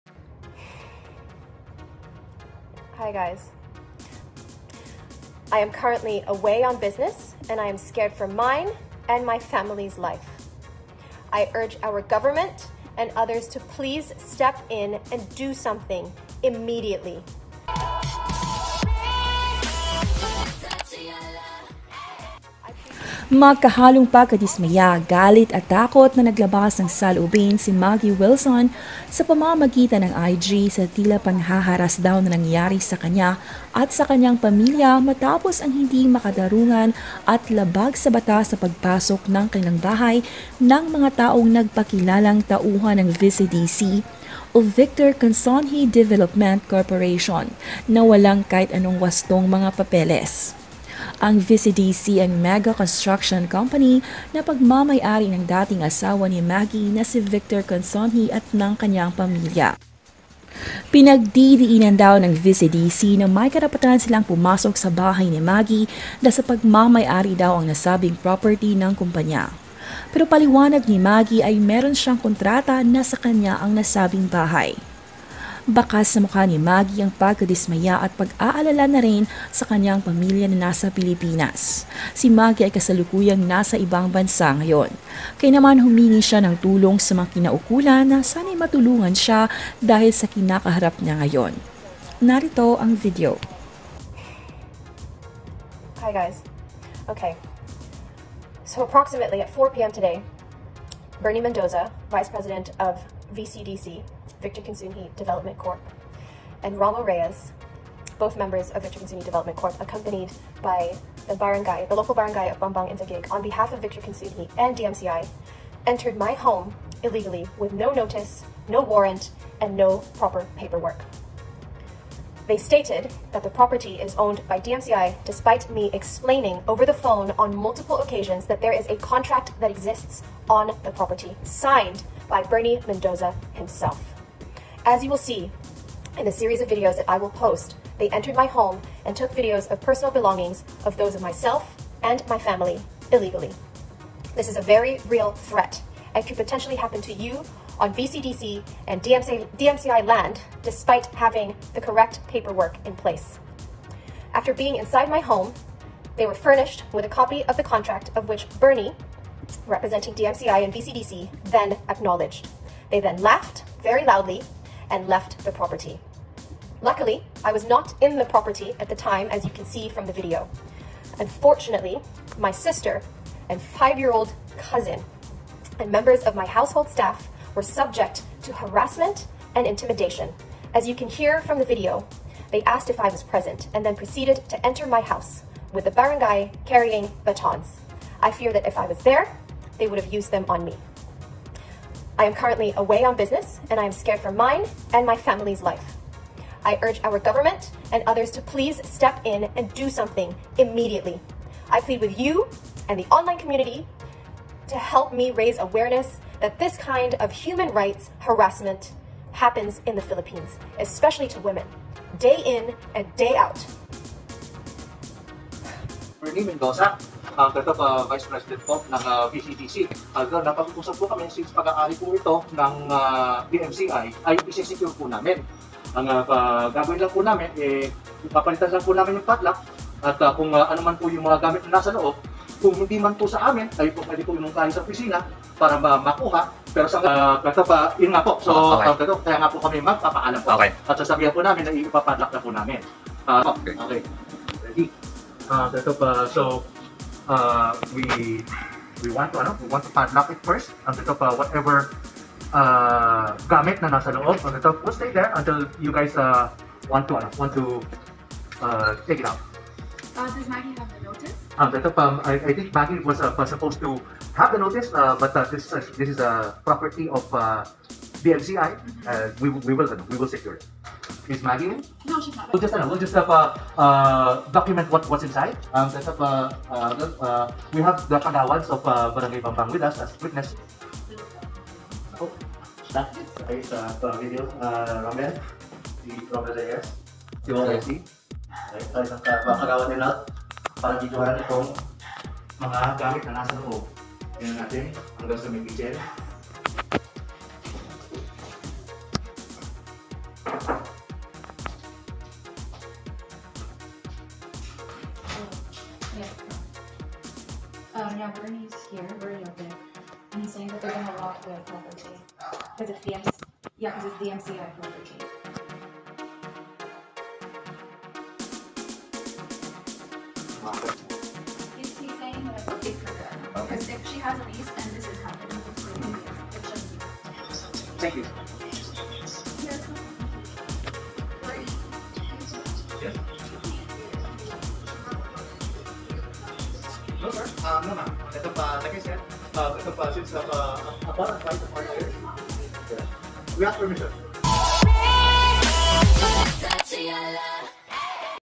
🎯KAWAWA!MAGGIE WILSON HUMIHINGI ng TULONG SA PUBLIKO MATAPOS ITO GAWIN ng TAUHAN ni VICTOR CONSUNJI!
Hala! Grabe naman ang ginawa nila kay Maggie Wilson. Bahay Pinadlock na!Maggie Wilson may nilabas na video. Panoorin..